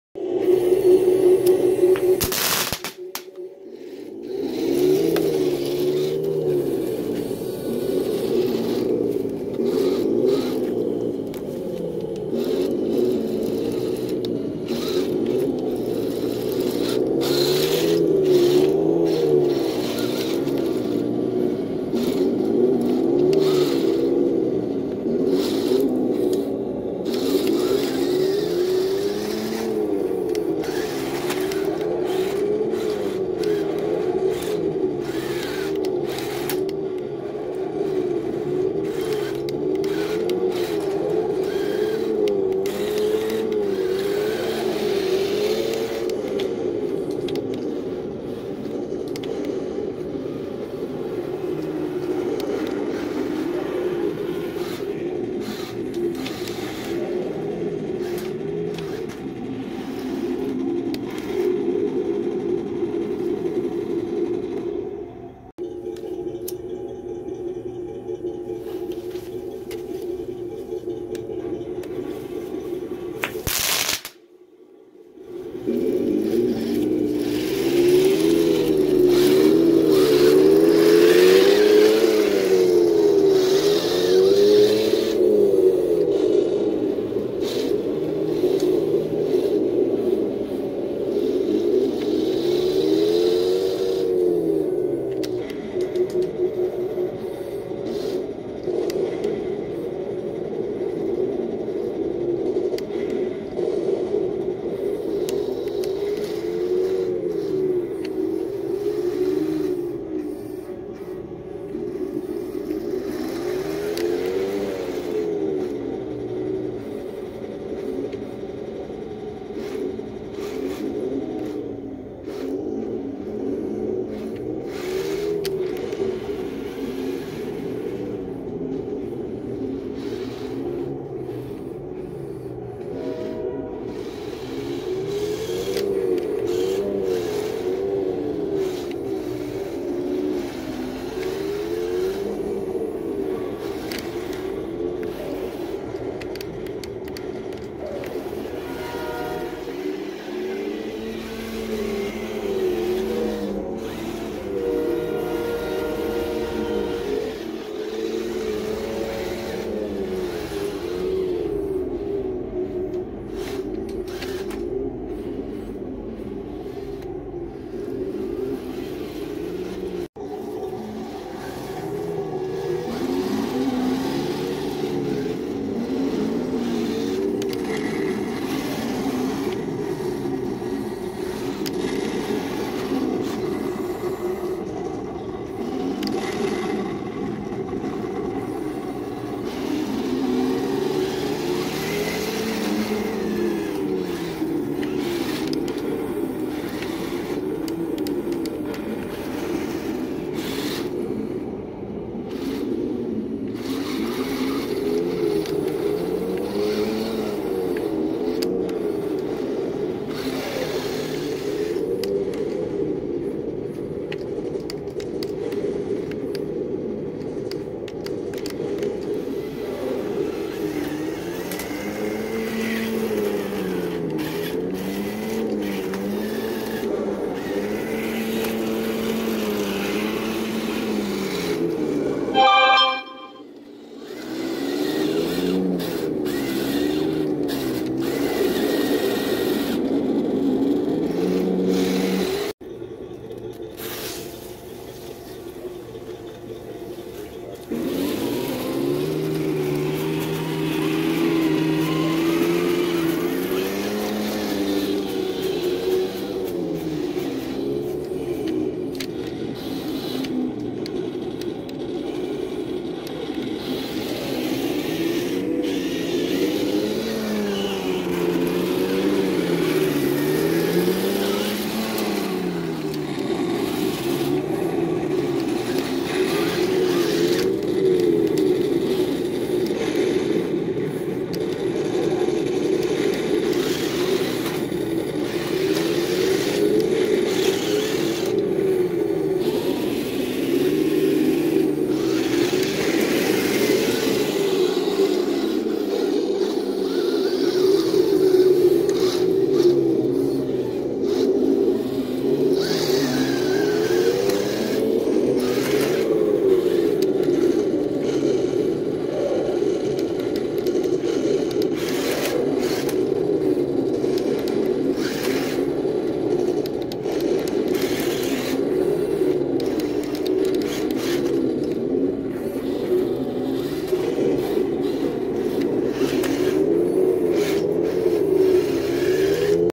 Tank Duel !!RcPart 1
As the techs expand the nite battles goes on
enemy forces meet face 2 face on the battle field !

Happy Holidays to All From V&b speed ☃️👋🇨🇦🇧🇷🇩🇪🇳🇱🇩🇰🇮🇳🇺🇲🚀🚀🇲🇽

For V&b Speed use only 🛑

Henglong Rc Battle Tanks 1/16 Scale